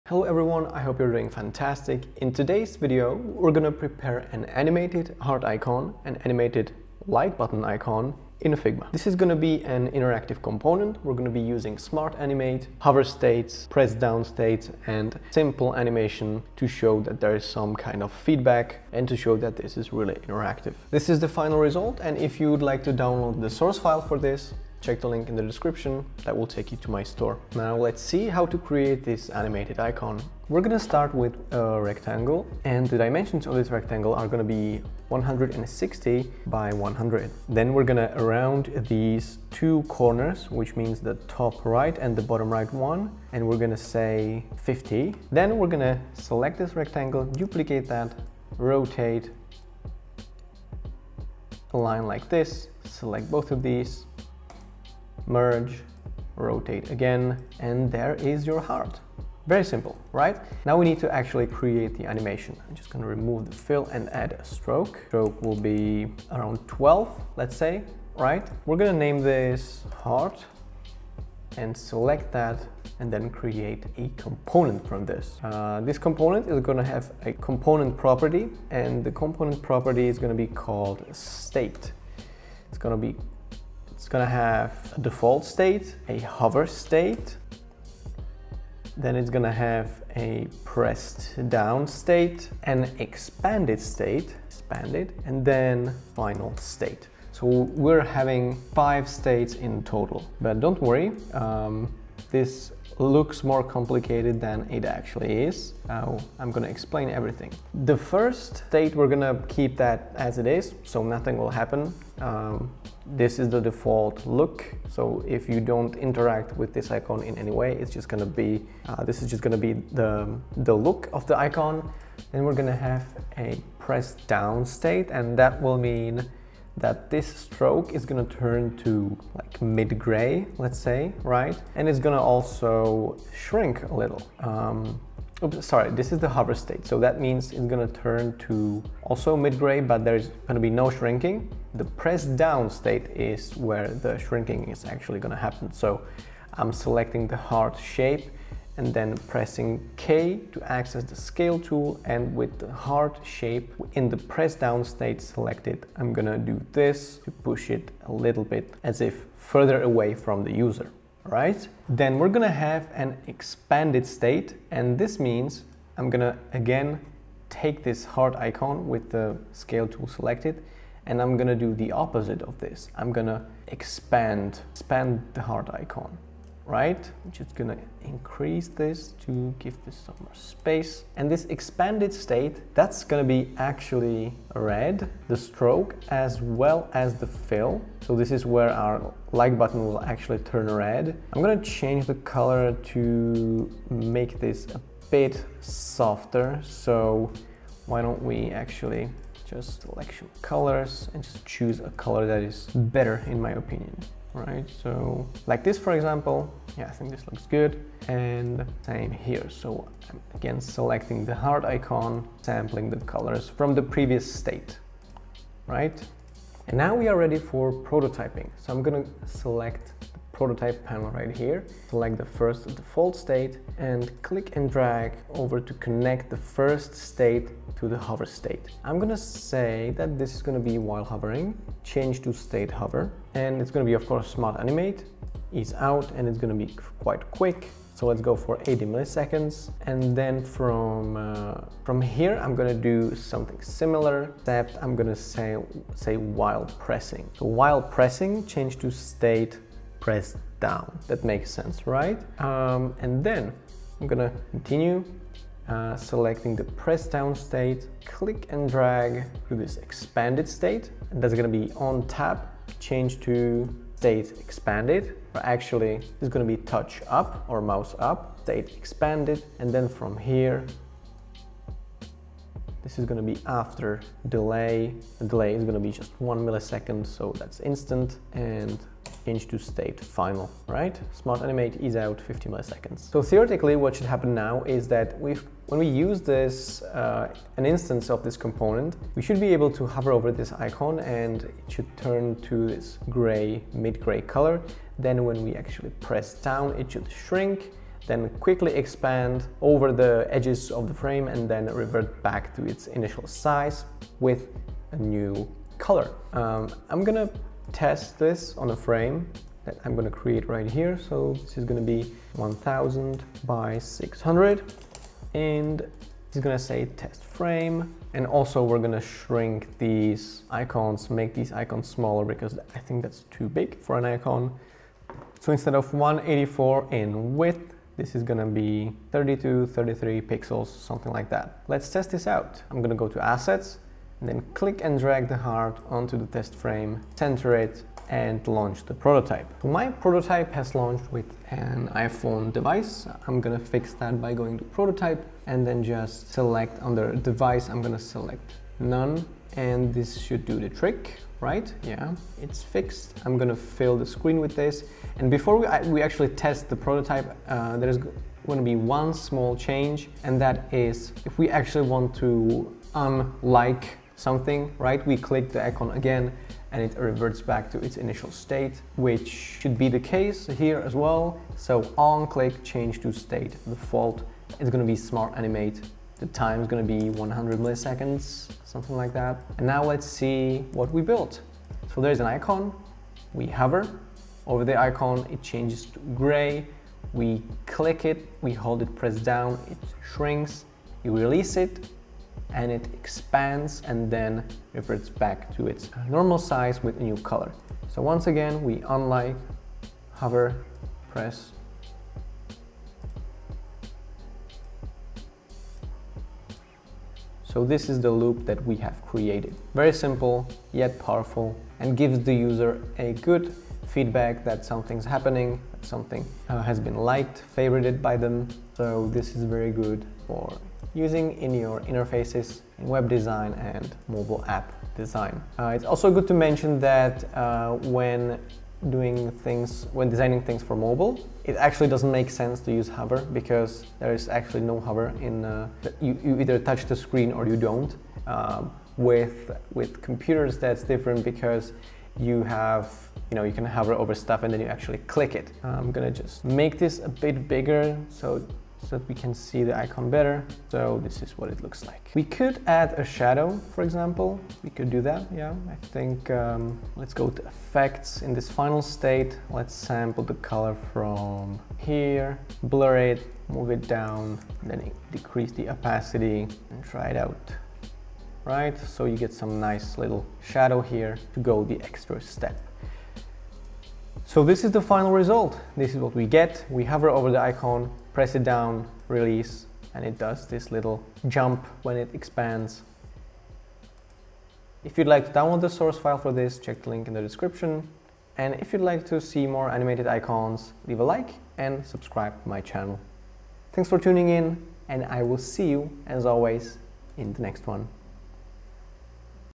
Create an Animated Like (Heart) Button in Figma
This Figma interactive prototyping tutorial will be focused on creating an animated like button represented by a simple heart icon. We will be using smart animate within a Figma component to achieve this result. We'll also create a prototype to test the final outcome.

Topics: figma, inteactive prototypes, interactive components, button, figma ux ui design